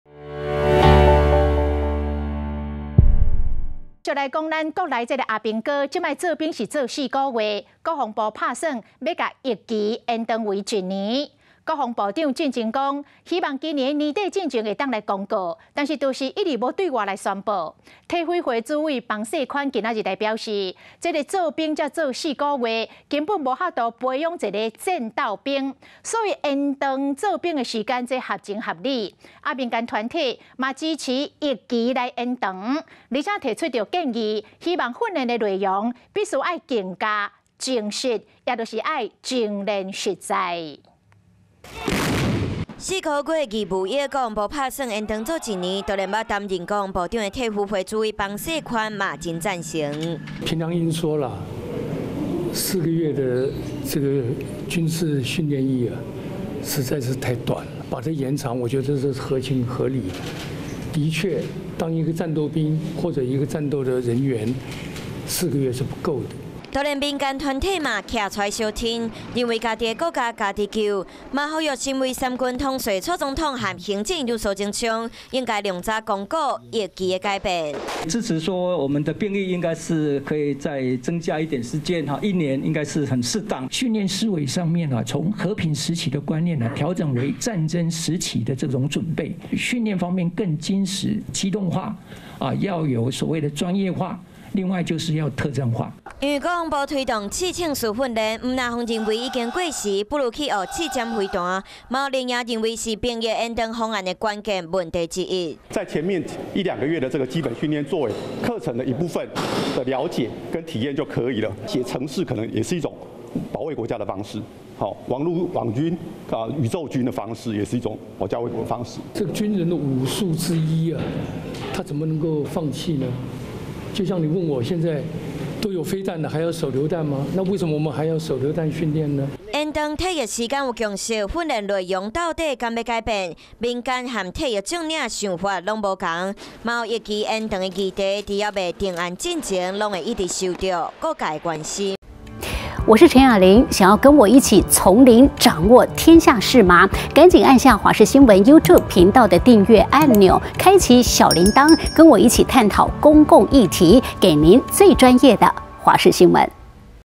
挺役期延長 馮世寬:4個月太短.延長合情合理｜華視台語新聞 2022.12.19
國防部打算將4個月的義務役役期延長為1年，日前國防部長說希望年底前公告，只是遲遲還沒對外宣布。退輔會主委馮世寬今(19)日表示，4個月時間無法培養一個戰鬥兵，延長合情合理，而民間團體不只希望政府盡快公告，也支持役期延長，但訓練內容必須要更精實，現行的刺槍術不是每個軍種都要學，寫程式的網軍也可以保家衛國。